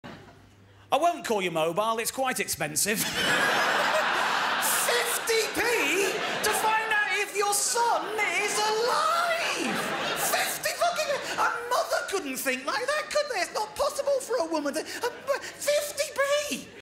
Dave Gorman 50p
Mattaf1990 in audience watching Dave Gorman Standup Live